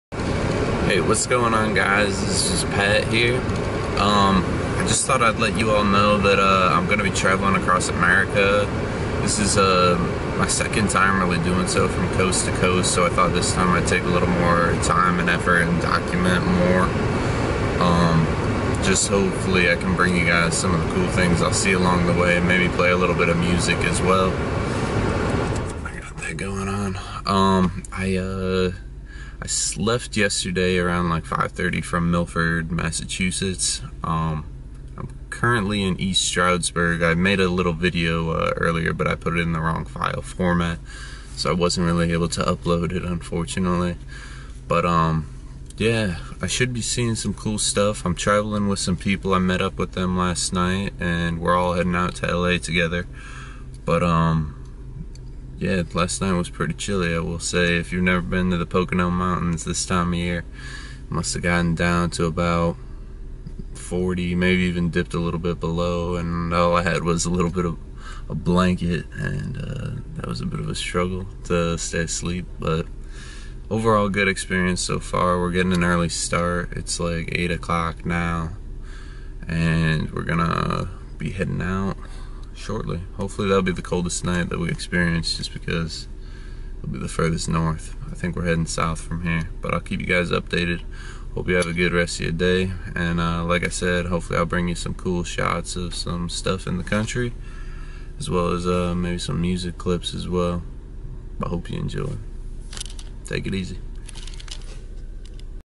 Traveling across America part 1
I'm going to be taking the next couple weeks to drive across the USA. I thought I would try and document what was going on along the way and try and bring some cool stuff to see. I started off in Milford Massachusetts and am hoping to end up in Los Angeles California. I am traveling with some friends so you will probably see them in future videos!